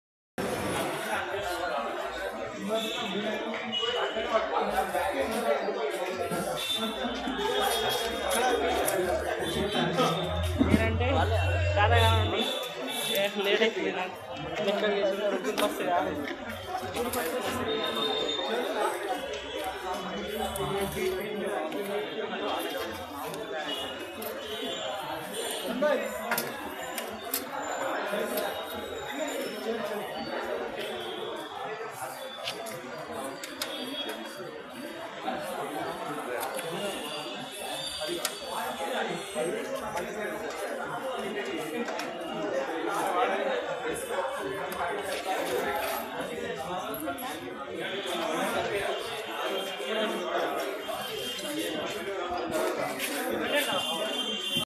Free Food by Hare Krishna
Free Food by Hare Krishna, this is the great work done by them. God has given opportunity to them. They are distributing free food in Govt Hospitals and rs 5/- in schools. The food is excellent like thirupathi food.